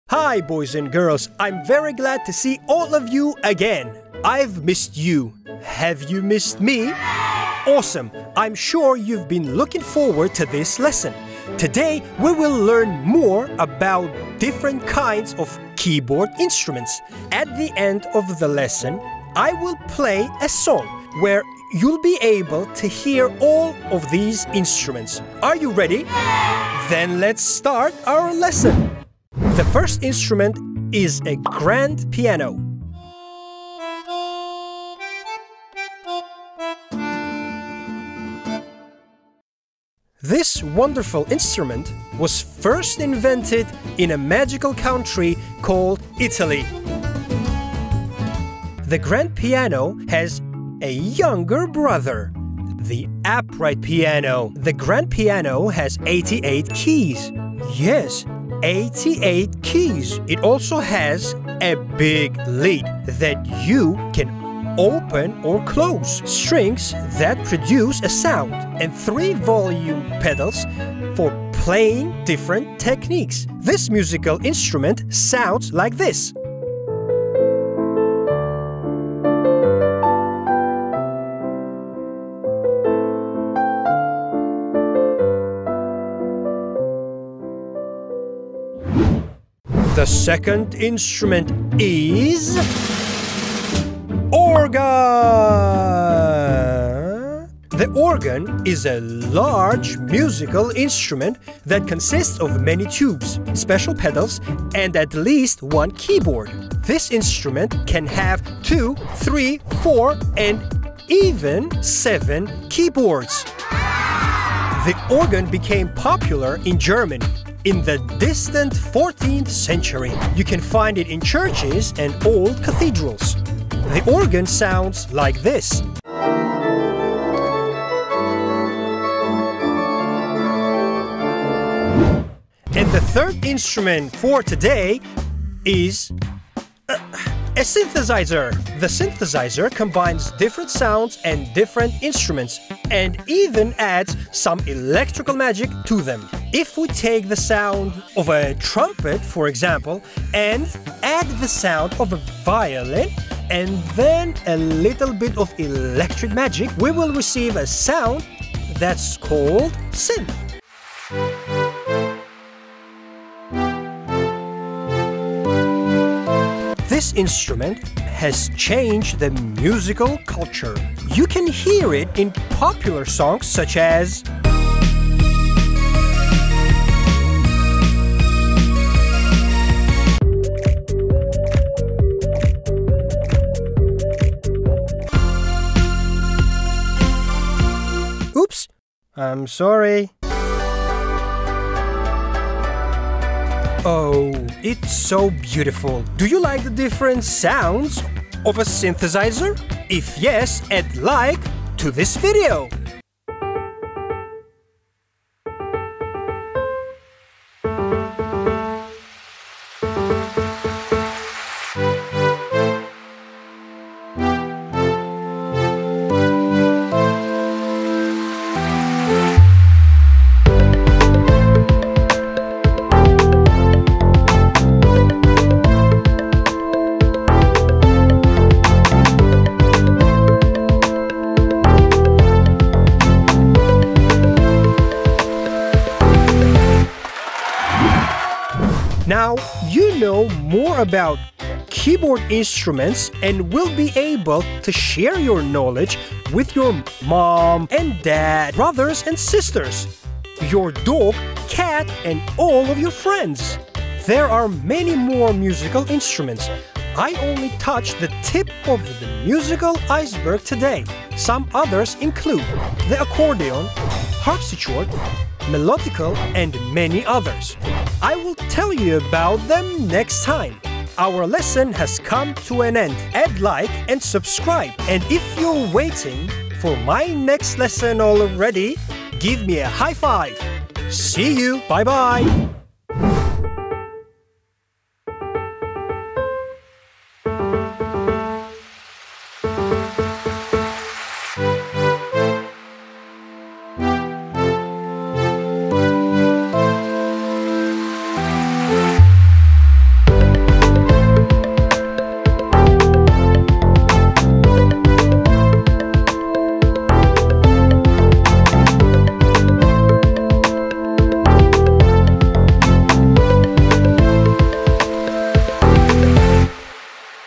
Variety of keyboard instruments. What do grand piano, organ and synthesizer sound like?
Today we will learn about different kinds of keyboard instruments.
For example, a grand piano, organ, synthesizer and many other musical keyboards. We will also learn a melody that can be easily played on a synthesizer.